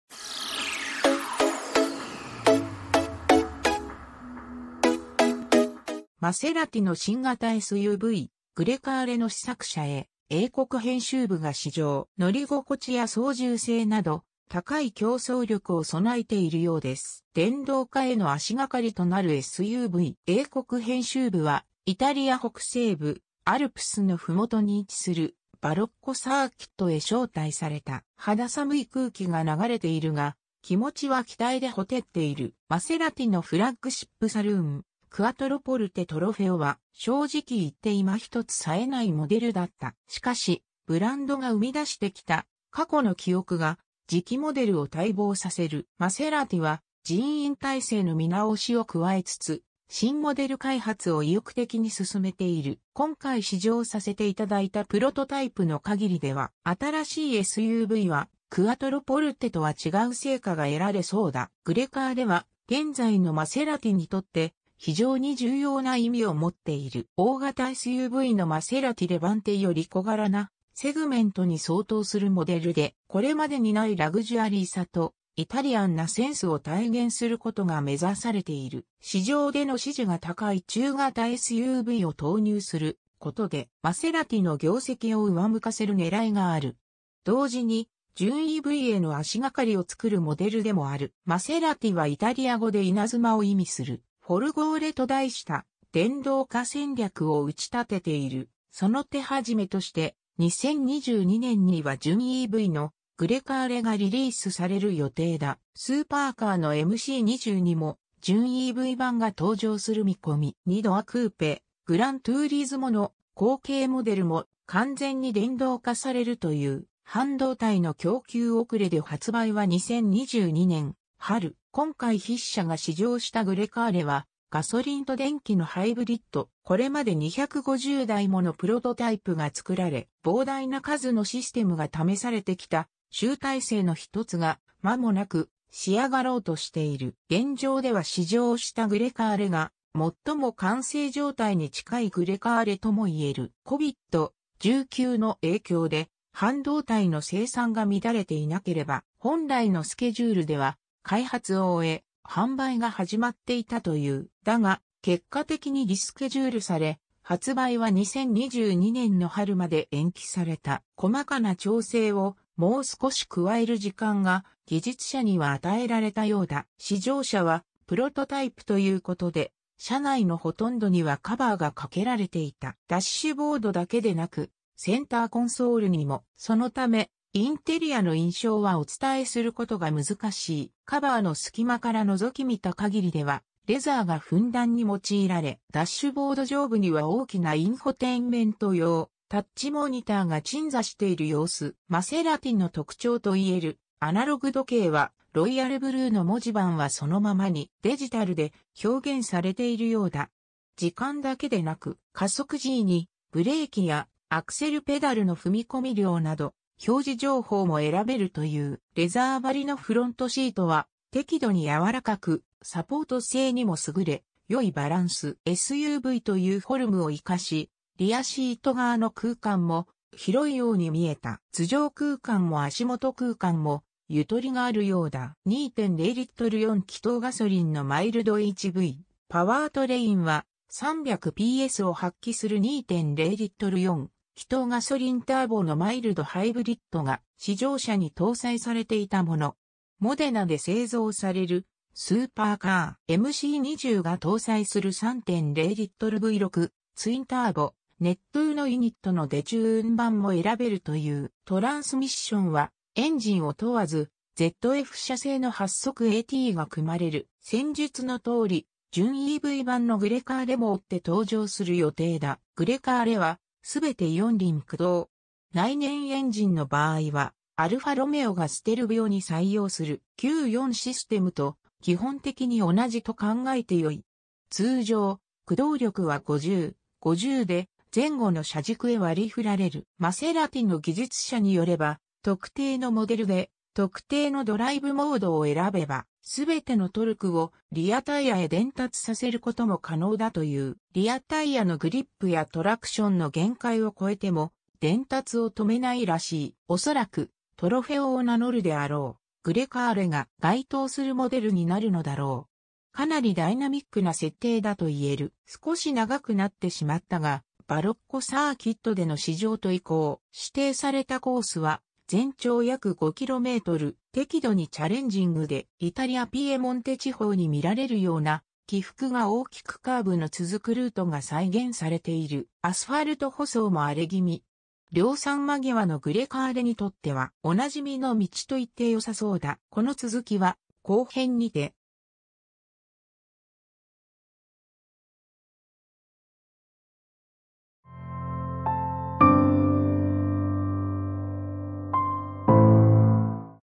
マセラティ・グレカーレ MHEV プロトタイプへ試乗 ステルヴィオの兄弟SUV 前編 | 車の話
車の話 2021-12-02

 マセラティの新型SUV、グレカーレの試作車へ英国編集部が試乗。  乗り心地や操縦性など、高い競争力を備えているようです。  電動化への足掛かりとなるSUV  英国編集部はイタリア北西部、アルプスの麓に位置するバロッコ・サーキットへ招待された。  肌寒い空気が流れているが、気持ちは期待で火照っている。  マセラティのフラッグシップ・サルーン、クアトロポルテ・トロフェオは、  正直いって今ひとつ冴えないモデルだった。  しかし、ブランドが生み出してきた過去の記憶が、次期モデルを待望させる。  マセラティは、人員体制の見直しを加えつつ、新モデル開発を意欲的に進めている。  今回試乗させていただいたプロトタイプの限りでは、  新しいSUVはクアトロポルテとは違う成果が得られそうだ。  グレカーレは、現在のマセラティに